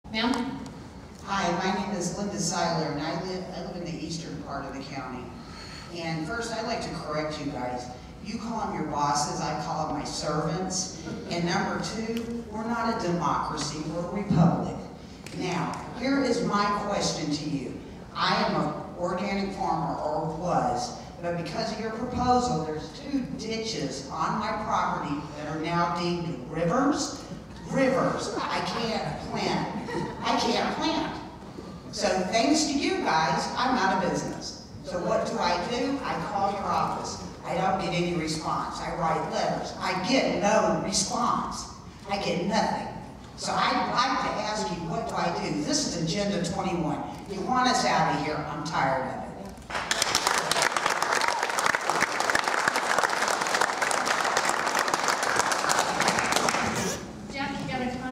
Sheeple2 or Linda at a meeting-Telling Government off First go round
The wild olympics project is Agenda 21-Patty Murry and Norm Dicks want our land-People opposed wore Red-I didn't I wore a federalJack shirt because I was not taking there shit 90% were against this land grab-i have never had more fun-even the police were shaking our hands